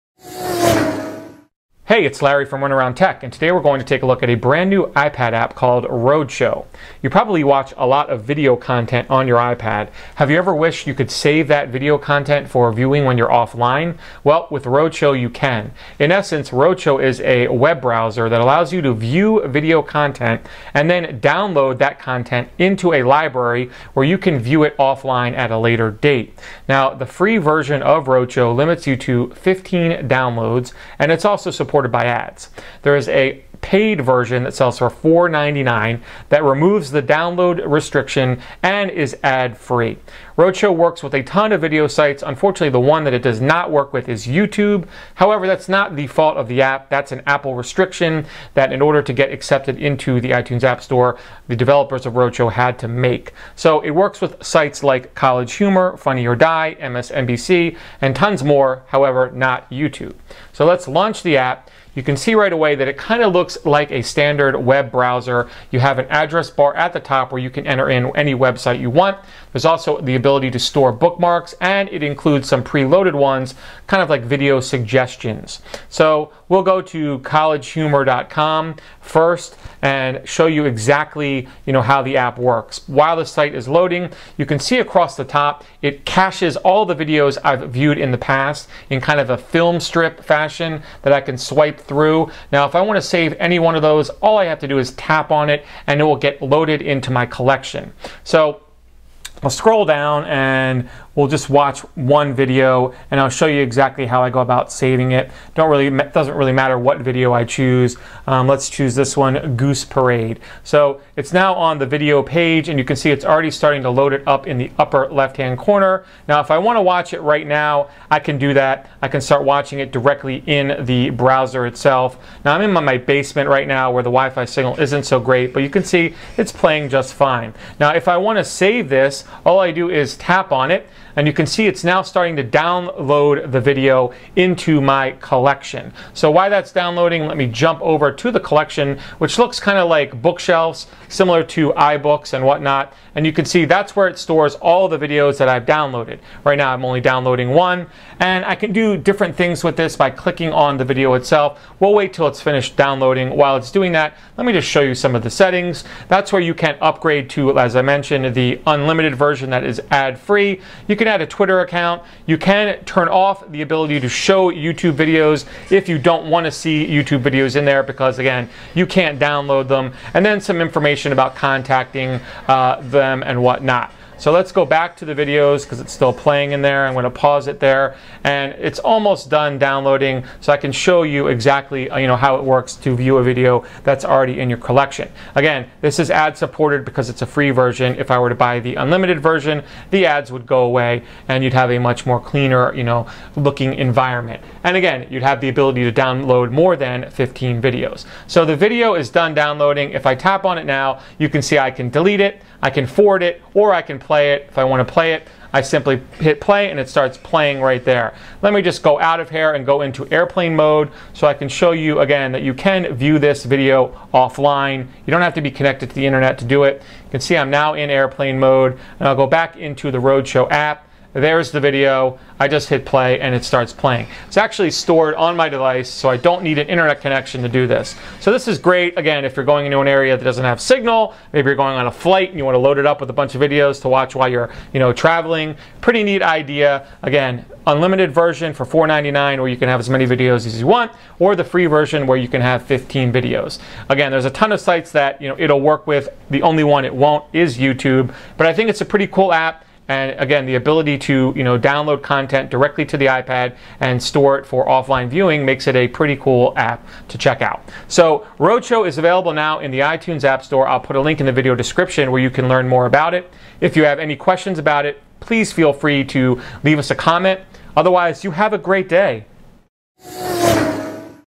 Roadshow for the iPad
In this video we review the iPad application Roadshow.